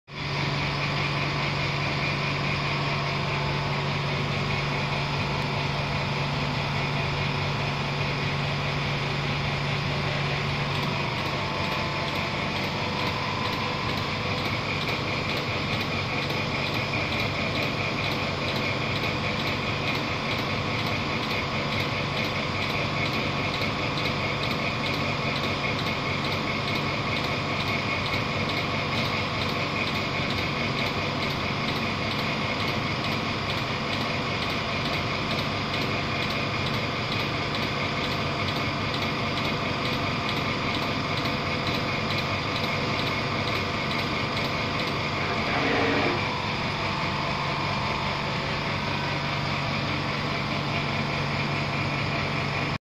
Y&D7300V Automatic dispensing machine with camera CCD
Y&D 7000V Series Automatic Vision Positioning Dispensing Robots which equipped with high performance CCD vision positioning system, visual dispensing route that can improve accuracy; According to different artifacts, choose suitable LED light source for lighting;

Skype:ydsales